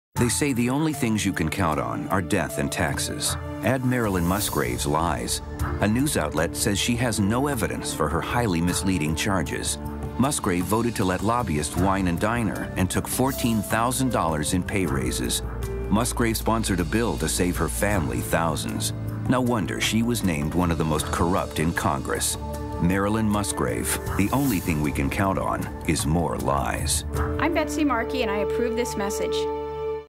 Marilyn Musgrave's Lies
Marilyn Musgrave continues to lie to and misrepresent the people of the Fourth Congressional District. Find out more about her actions in Congress and how they affect the people of Colorado.